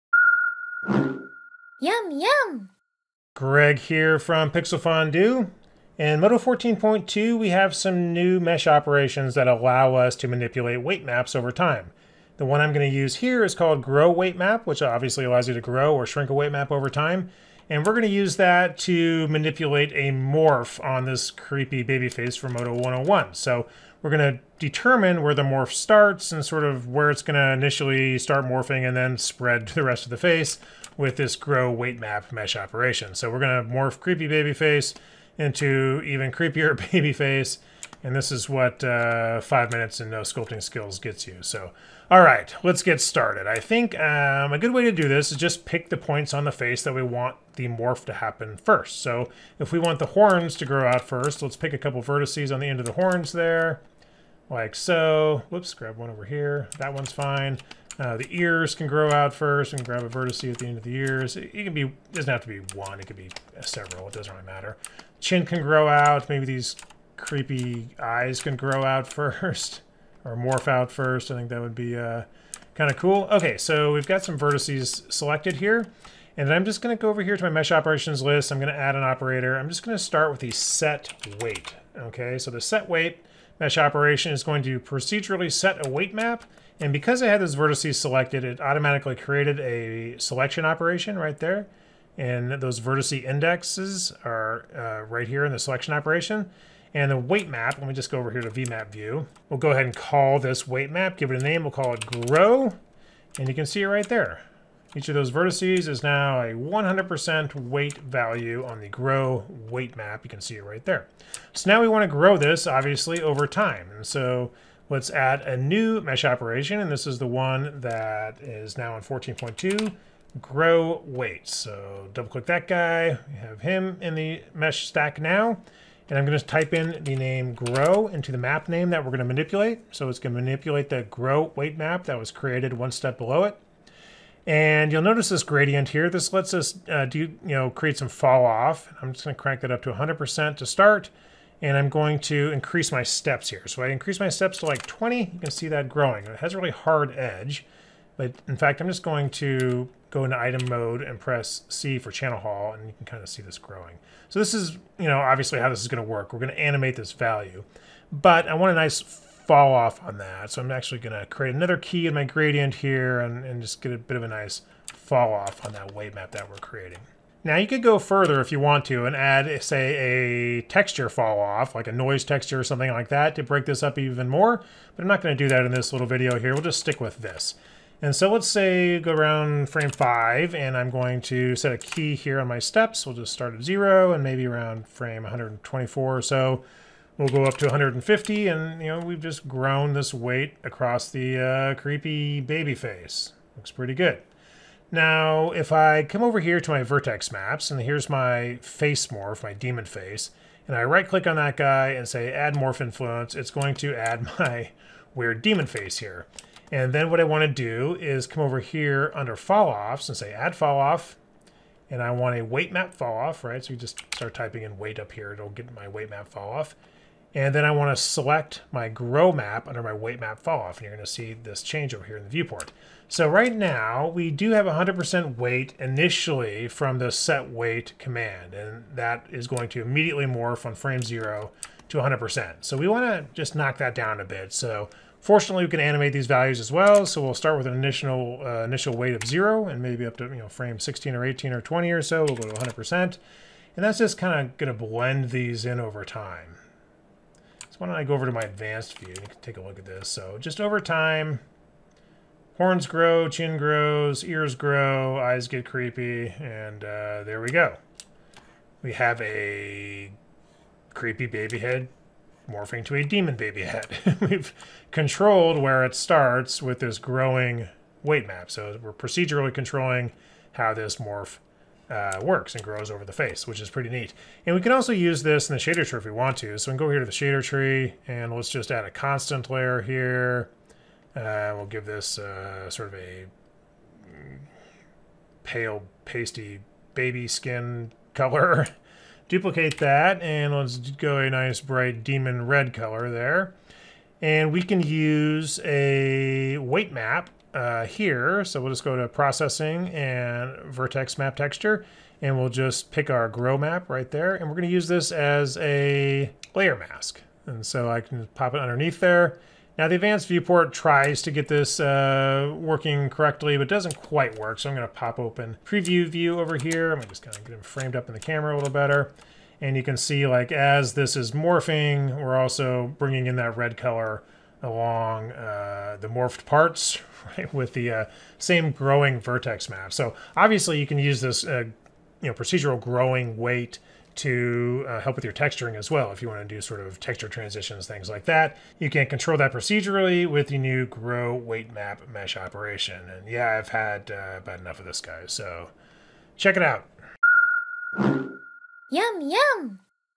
MODO | Morphin with a procedural weight map
The Grow Weight MeshOp can give you control over your deformations.